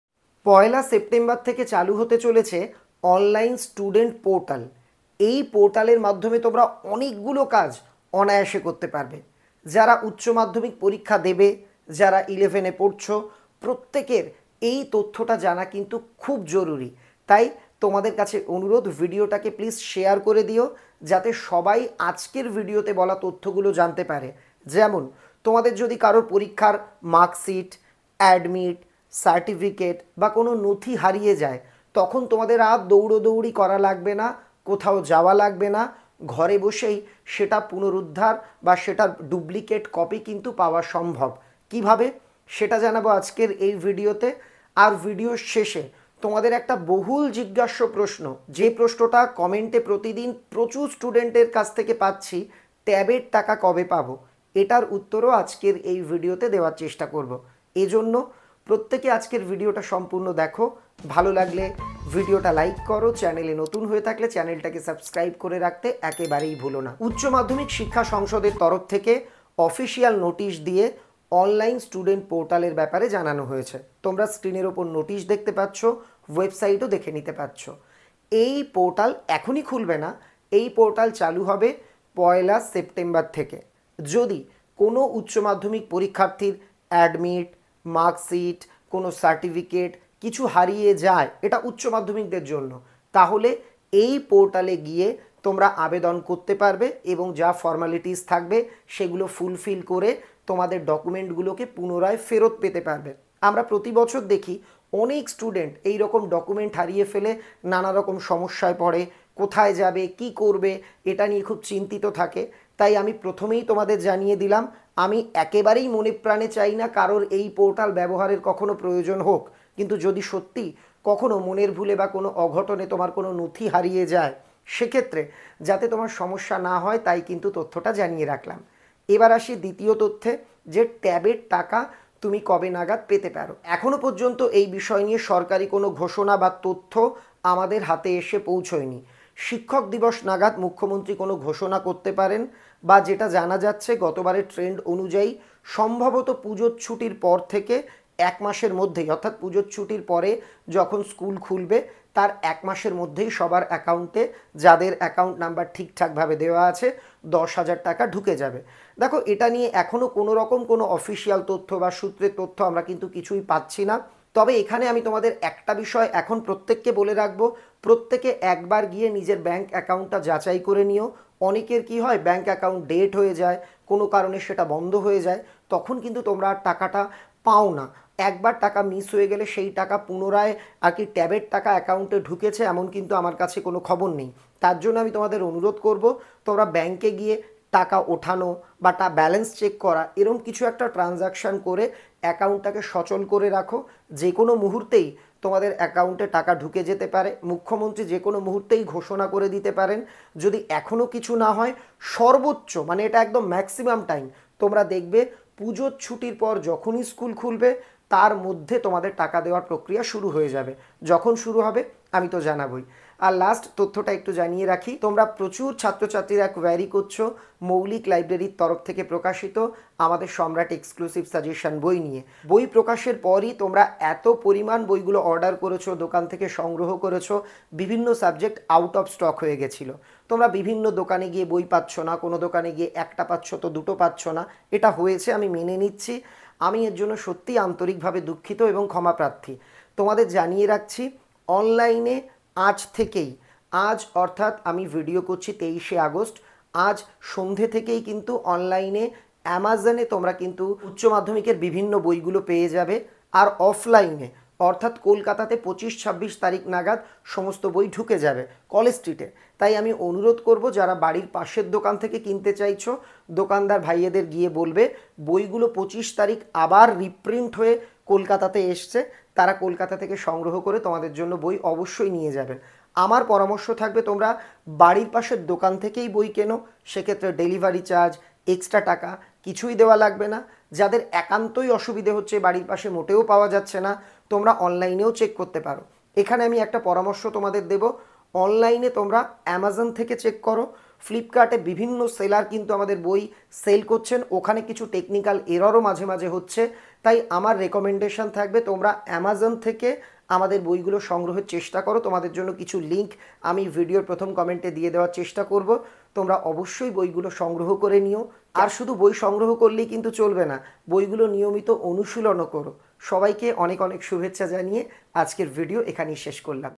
চালু হচ্ছে স্টুডেন্ট পোর্টাল ! হারানো ডকুমেন্টস ফেরত পাবে ঘরে বসেই || ট্যাবের টাকা কবে দেবে ?
উচ্চমাধ্যমিক পরীক্ষার্থীদের জন্য গুরুত্বপূর্ণ কিছু আপডেট

🔥 Instagram id & Twitter ID : SamratWB